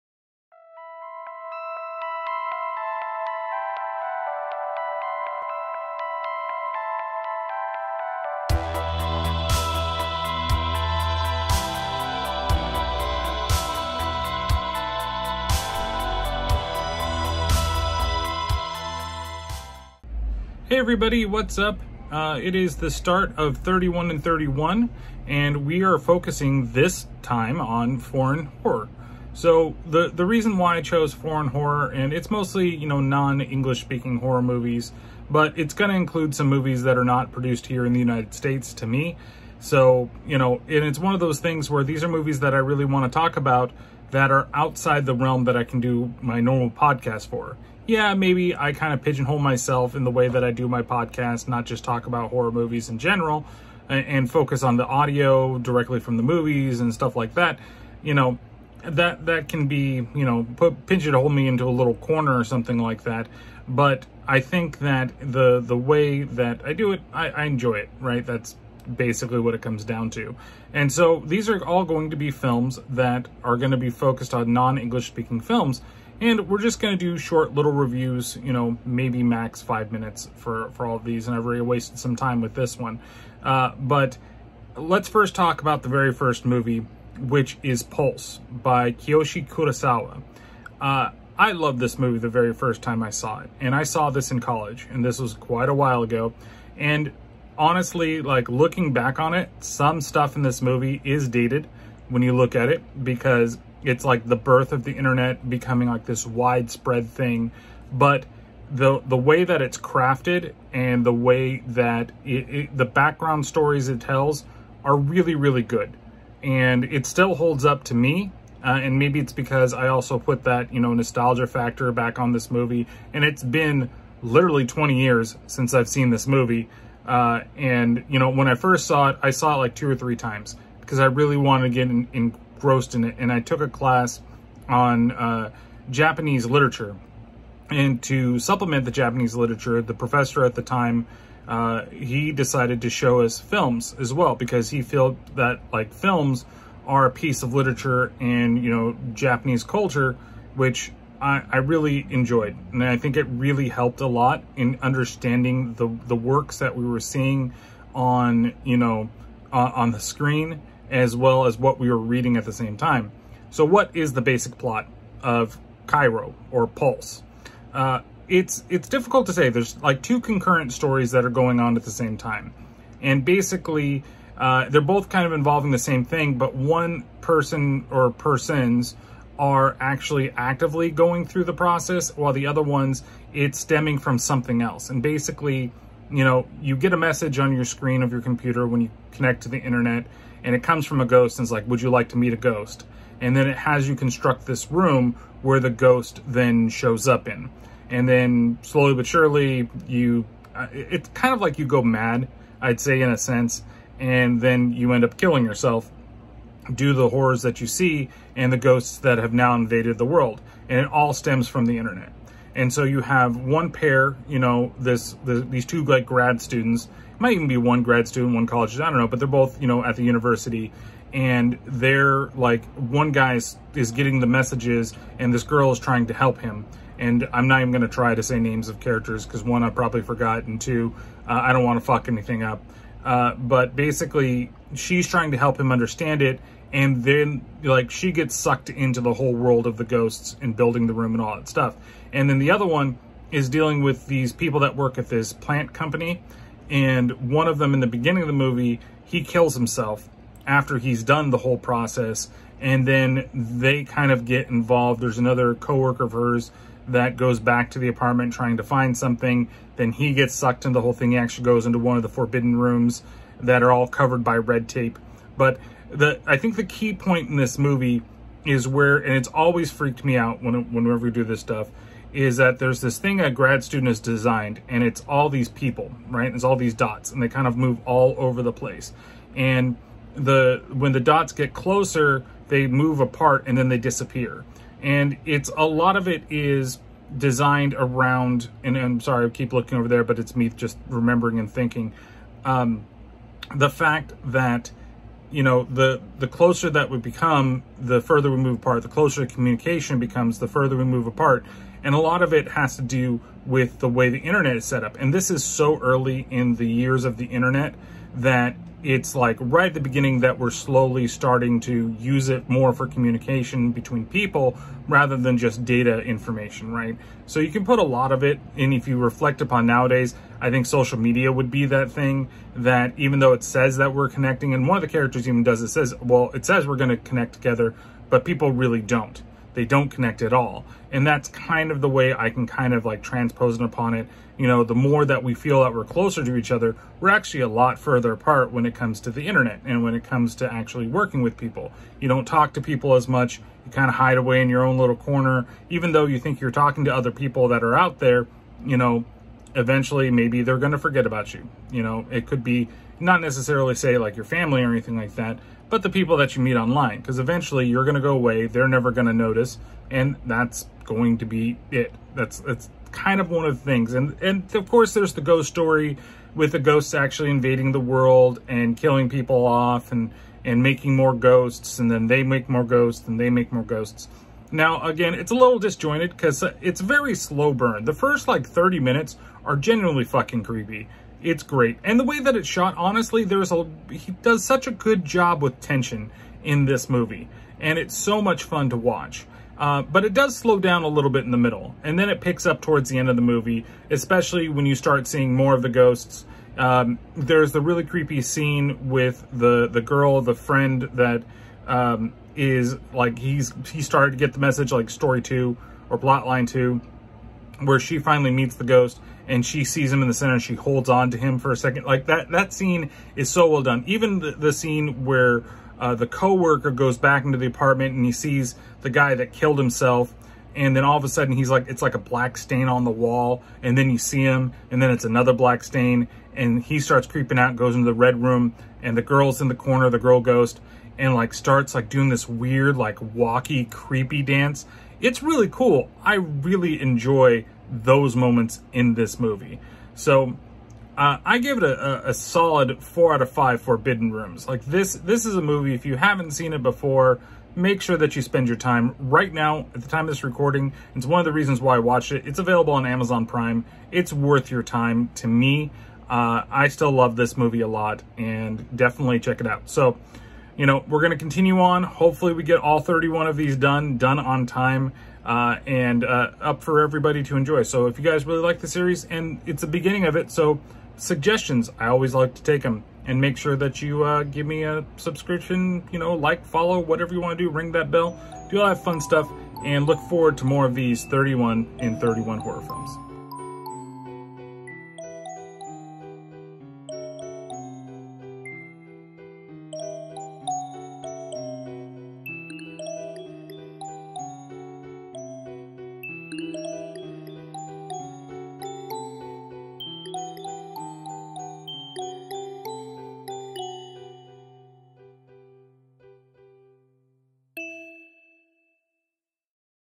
Foreign Horror Day 1: Kairo (Pulse)
This year for the 31 in 31, I am looking at international horror films. For day 1 we take a look at Kiyoshi Kurosawa’s Kairo!

Check out the Terrible Terror Store On TeePublic! The new Corn Tree design is now available: